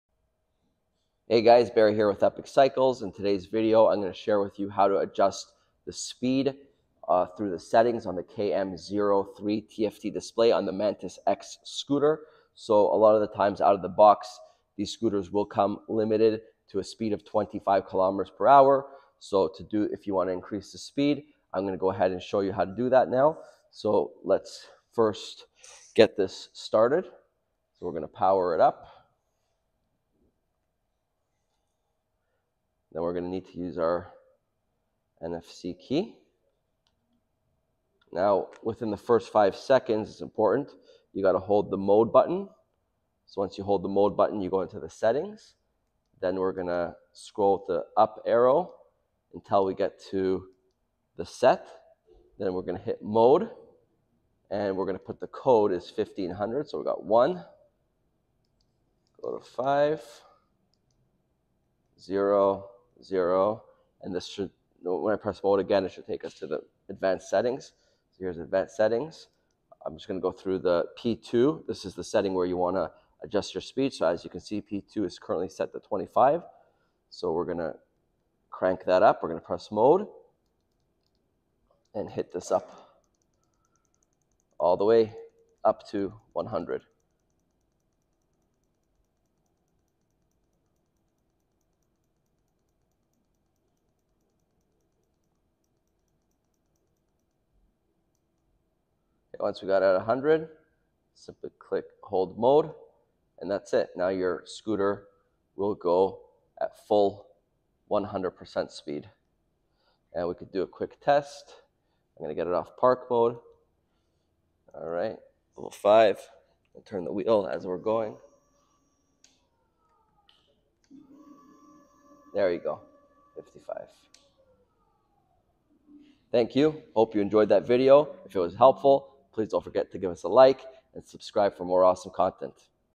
Unlocking and adjusting speed settings on the Kaabo Mantis X Scooter
In this video Barry shows you how to unlock and adjust your speed settings on the Kaabo Mantis X electric scooter through the KM03 TFT display.

Steps:
1. Power on display
2. Unlock with NFC key
3. Within 5 seconds of unlocking hold the M button to get into settings mode
4. Using the up arrow, scroll to Set and enter pass code 1500 to enter advanced settings
5. Got to P2, and adjust your speed settings from 0 - 100